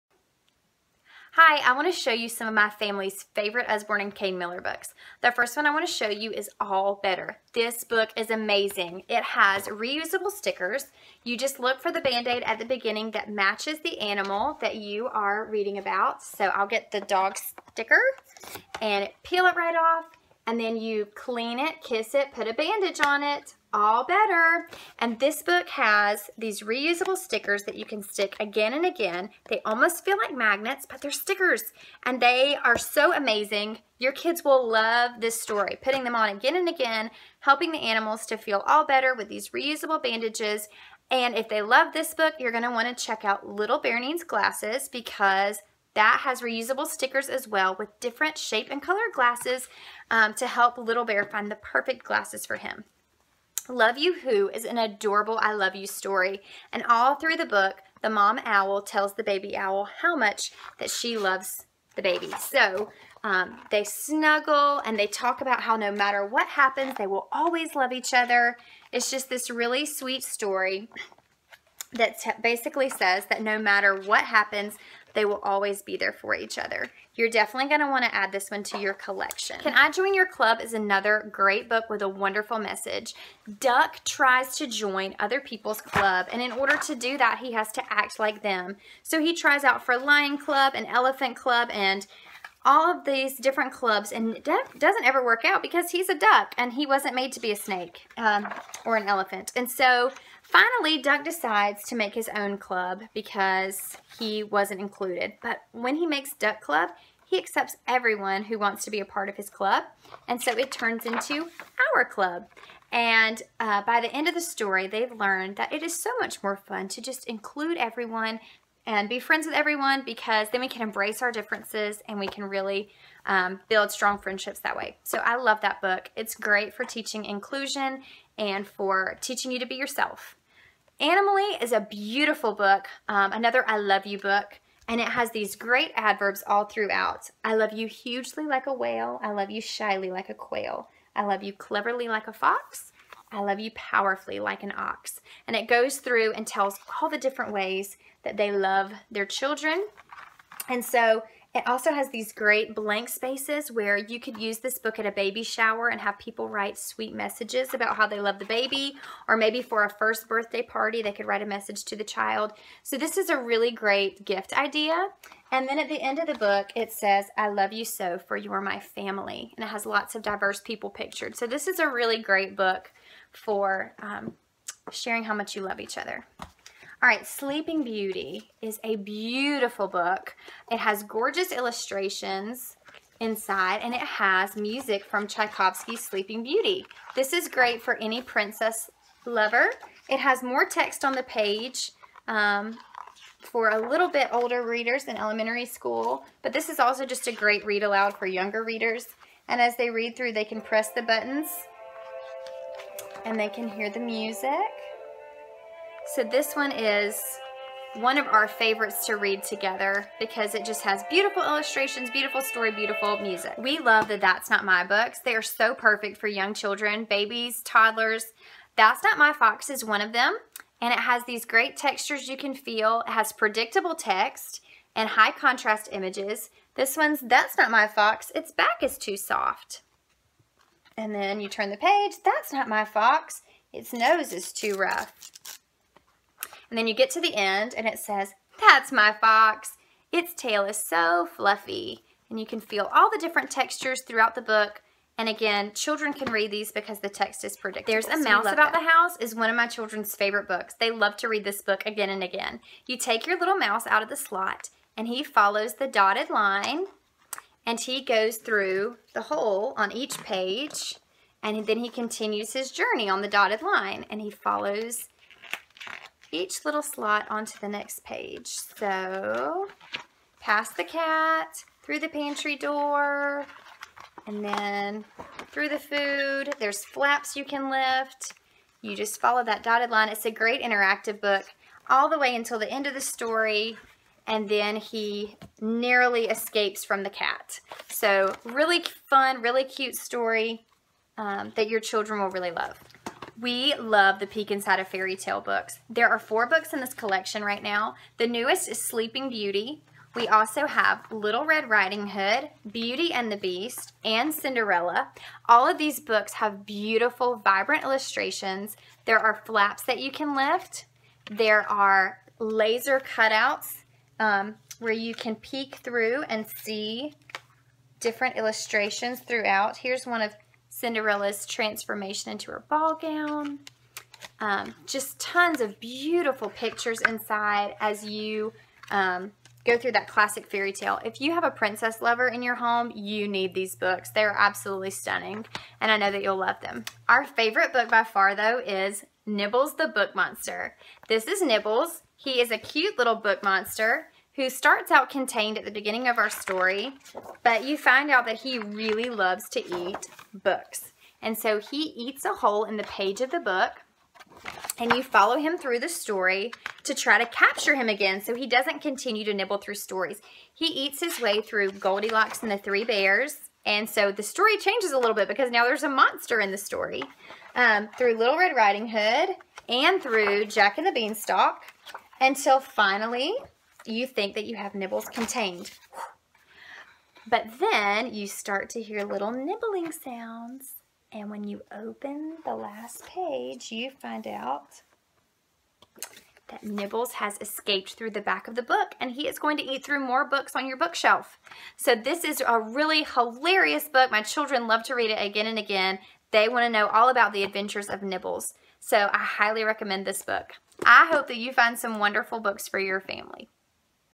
Zeigler Family Favorites
These are some of my family's favorite Usborne & Kane Miller books!